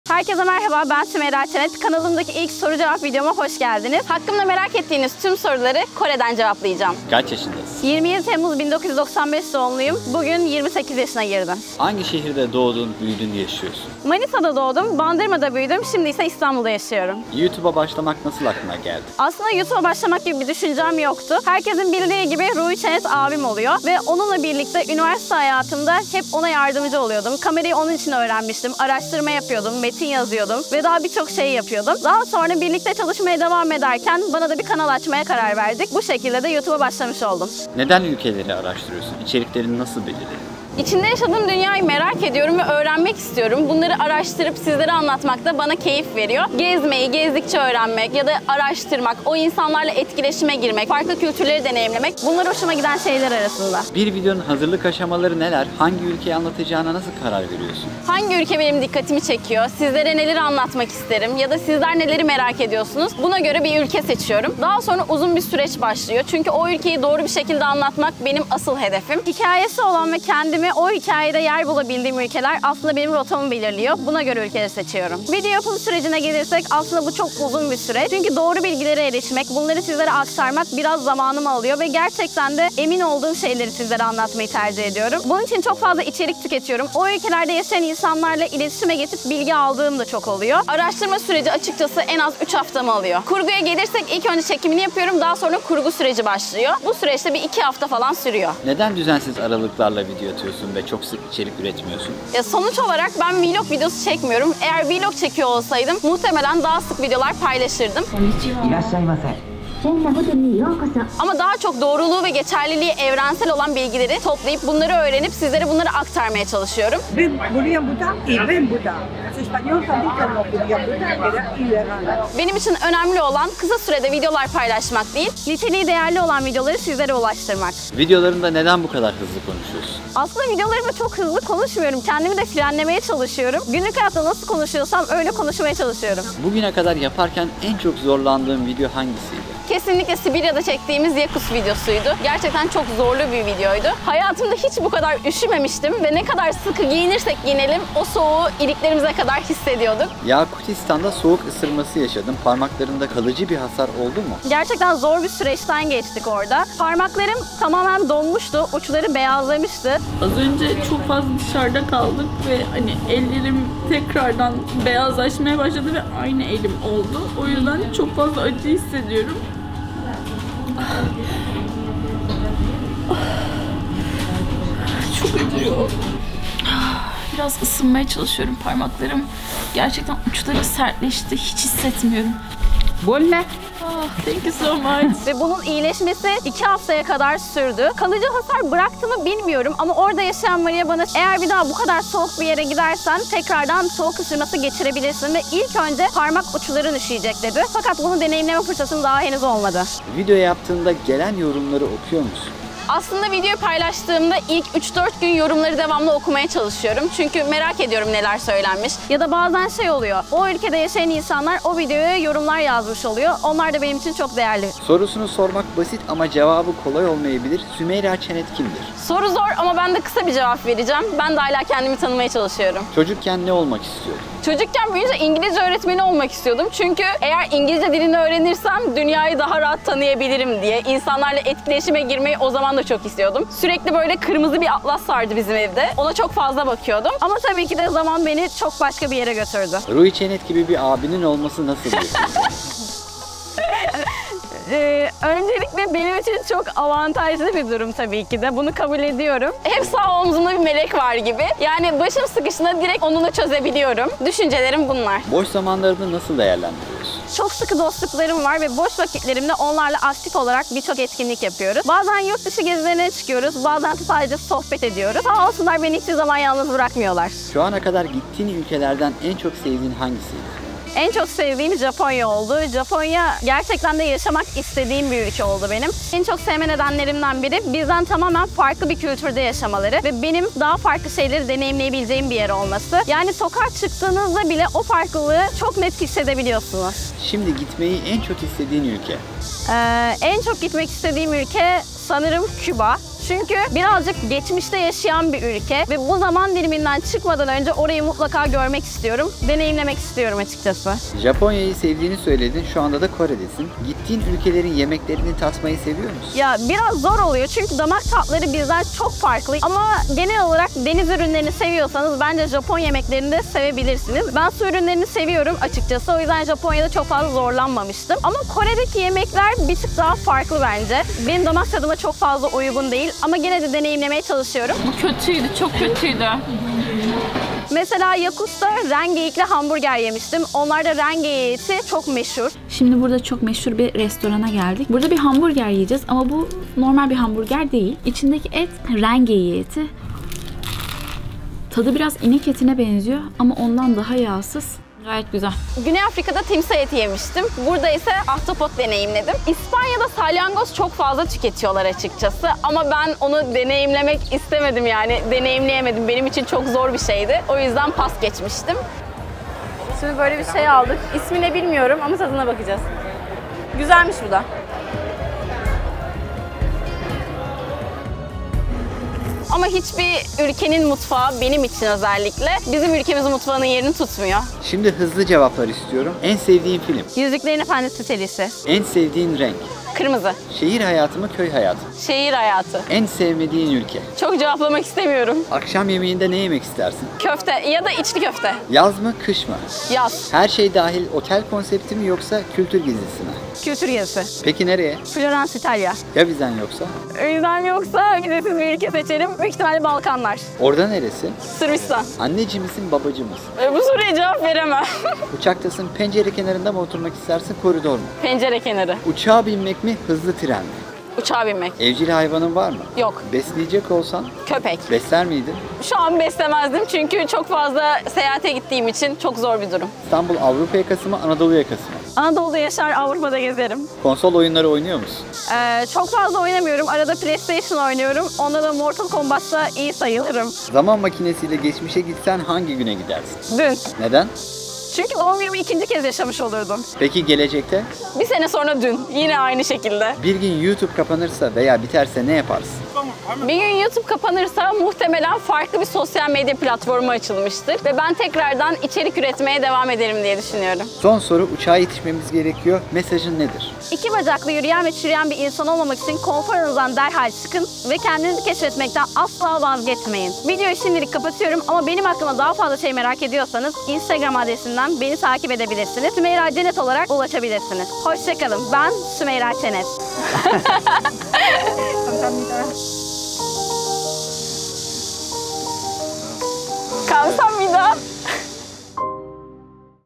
HAKKIMDA BİLMEDİĞİNİZ 33 GERÇEK
Herkese merhaba ben Sümeyra Çenet. Geçtiğimiz aylarda sizlerden hakkımda merak ettiğiniz soruları sormanızı istemiştim. Gelin daha yakından tanışalım...